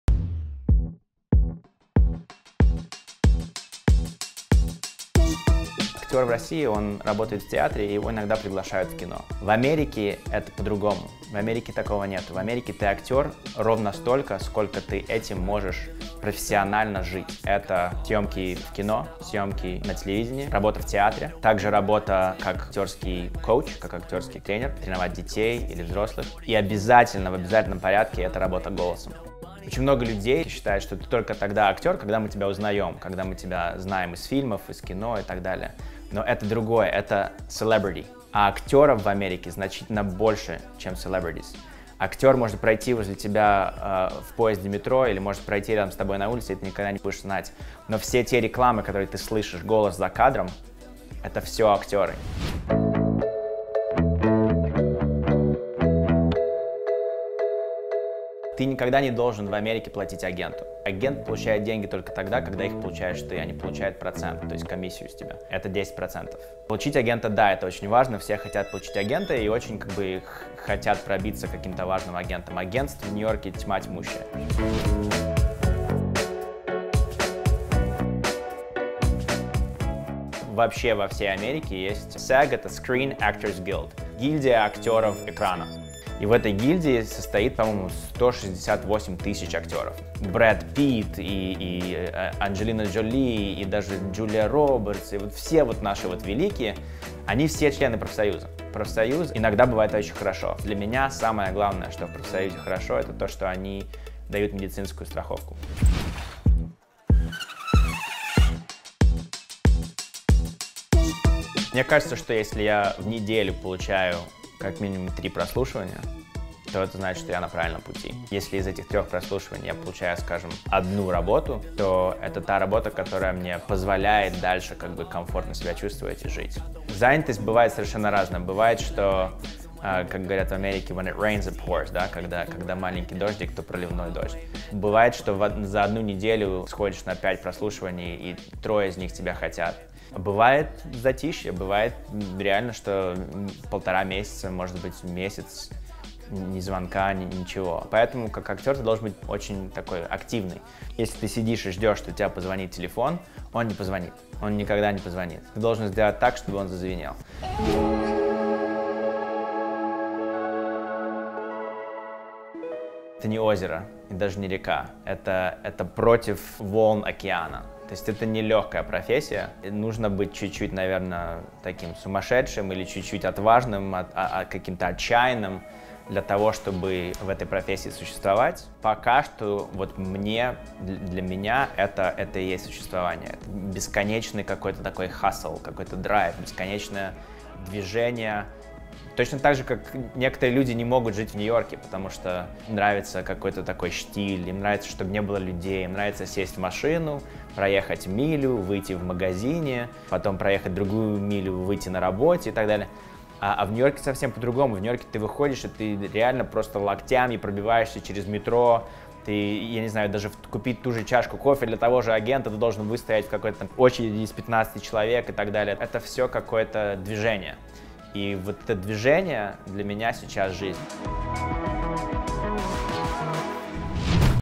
Профессия – актер
Как сделать карьеру актера в Нью-Йорке и что дает профсоюз актеров кино и телевидения